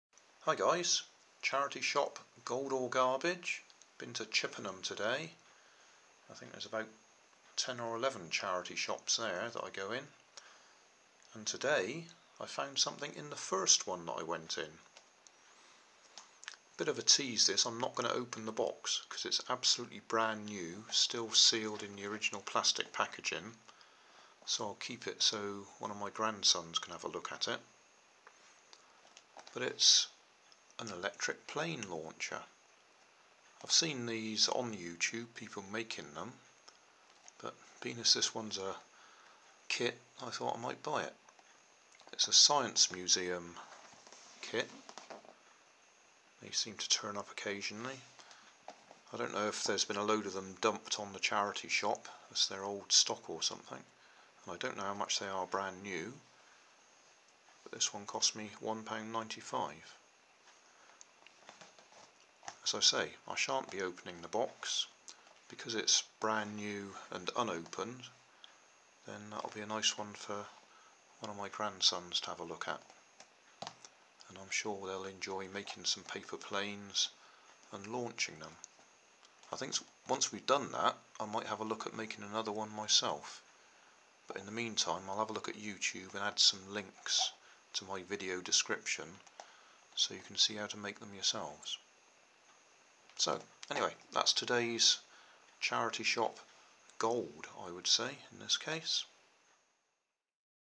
Charity Shop Gold or Garbage? Electric Plane Launcher
Brand new in it's original shrink wrap plastic so I shall let one of my grandchildren open this and maybe we will make some paper planes to test it. There are plenty of scratch built examples on YouTube and I have made my own Electric Plane Launcher